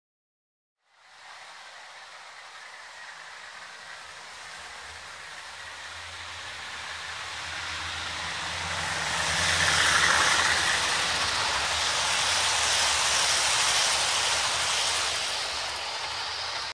Prototypes Together1
10203 and DP2 running together. 10203 has Legomanbiffo sound.